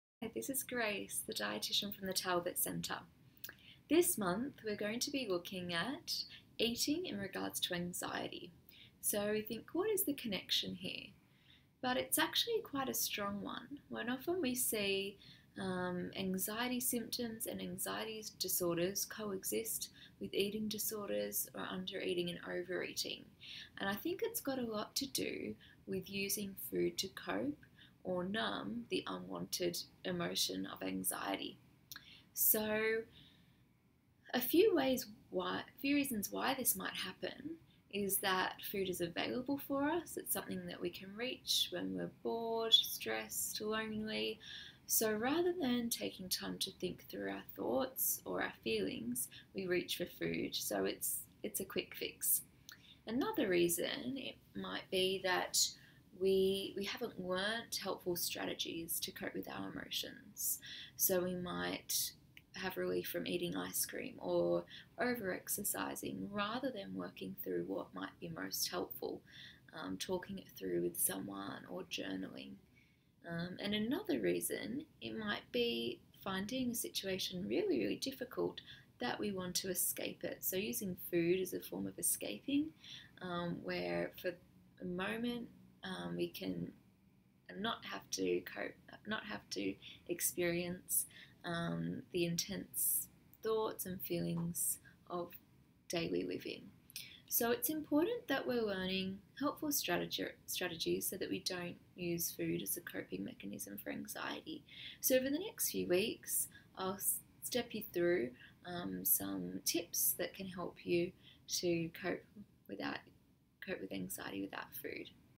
The Relationship Between Eating and Anxiety
The Talbot Centre Dietitian, Grace Russell, speaks about the relationship between feelings of anxiety and eating behaviour.